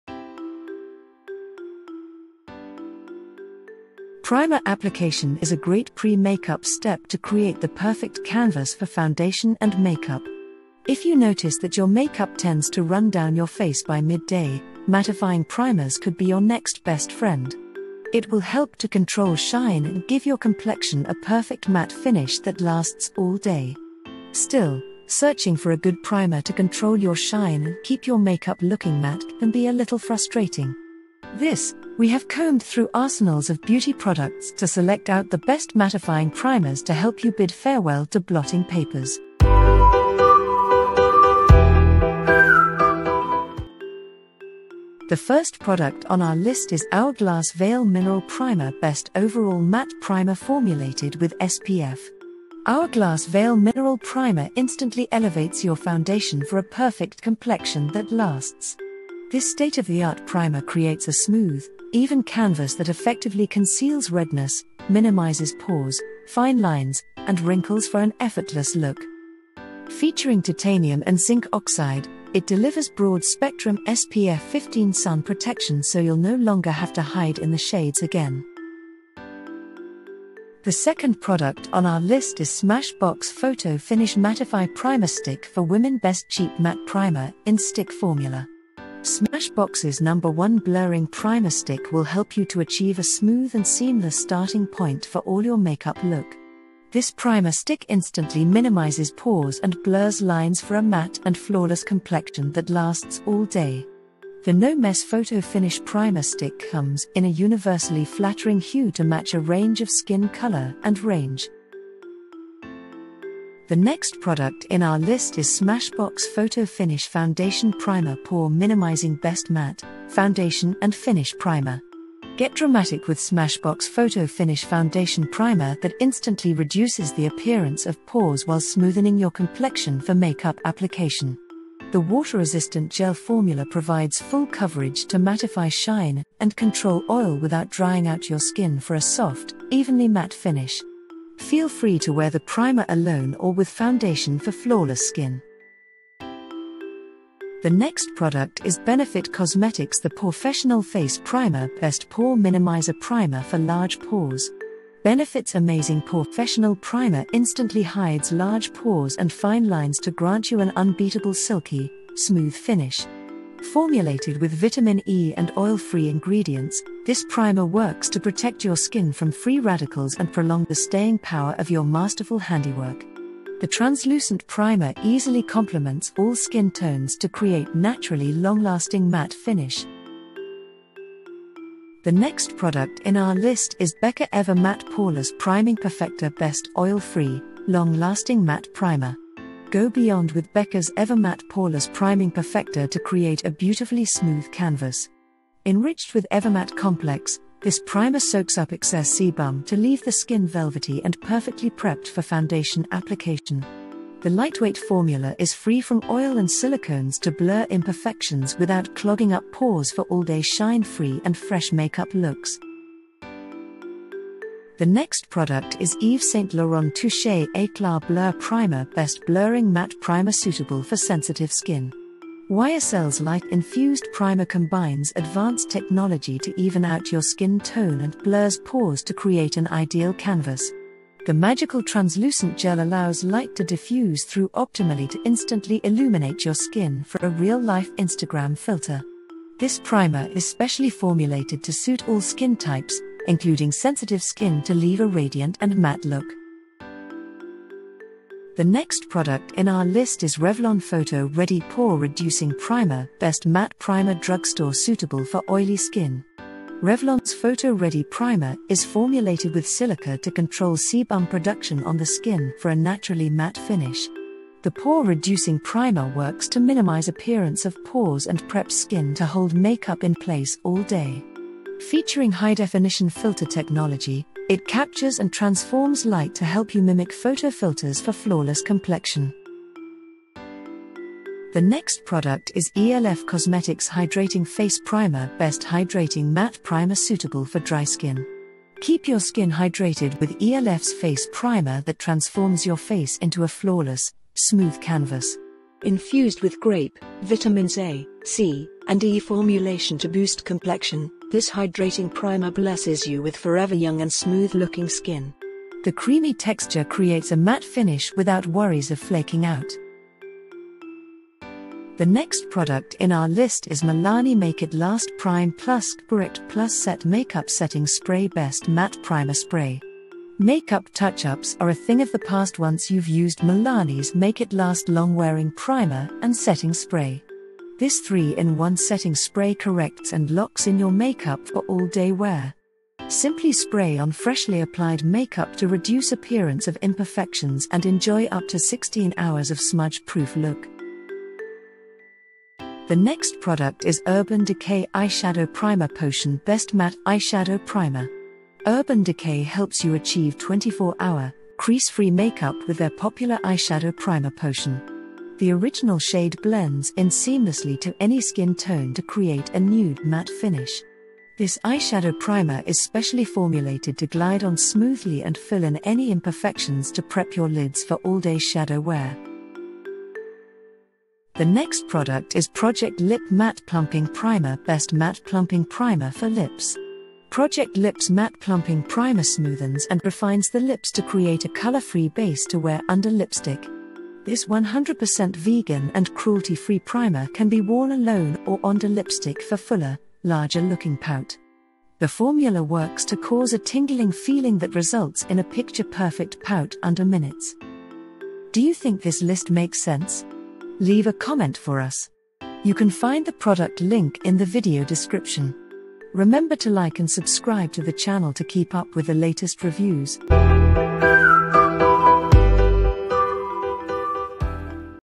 🌵 11 Best Matte Primers in Singapore
🌵 11 Best Matte Primers in Singapore | Lemongrass. Here are our editor picks:

00:00 Introduction

00:43 #1 - Hourglass Veil Mineral Primer Best overall matte primer - formulated with SPF (null)

01:18 #2 - SmashBox Photo Finish Mattify Primer Stick for Women Best cheap matte primer - in stick formula (null)

01:51 #3 - Smashbox Photo Finish Foundation Primer - Pore Minimizing Best matte, foundation and finish primer (SGD25.00)

02:25 #4 - Benefit Cosmetics The POREfessional Face Primer Best pore minimizer primer for large pores (null)

03:00 #5 - Becca Ever Matte Poreless Priming Perfector Best oil-free, long-lasting matte primer (SGD59.00)

03:32 #6 - Yves Saint Laurent Touche Eclat Blur Primer Best blurring matte primer - suitable for sensitive skin (null)

04:06 #7 - Revlon PhotoReady Pore Reducing Primer Best matte primer drugstore - suitable for oily skin (SGD15.08)

04:39 #8 - e.l.f Cosmetics Hydrating Face Primer Best hydrating matte primer - suitable for dry skin (SGD8.27)

05:09 #9 - Milani Make It Last Prime + Correct + Set Makeup Setting Spray Best matte primer spray (null)

05:40 #10 - Urban Decay Eyeshadow Primer Potion Best matte eyeshadow primer (null)

06:09 #11 - Project Lip Matte Plumping Primer Best matte plumping primer for lips (null)

06:37 Ending

Primer application is a great pre-makeup step to create the perfect canvas for foundation and makeup. If you notice that your makeup tends to run down your face by mid-day, mattifying primers could be your next best friend. It will help to control shine and give your complexion a perfect matte finish that lasts all day.
Still, searching for a good primer to control your shine and keep your makeup looking matte can be a little frustrating. This, we have combed through arsenals of beauty products to select out the best mattifying primers to help you bid farewell to blotting papers.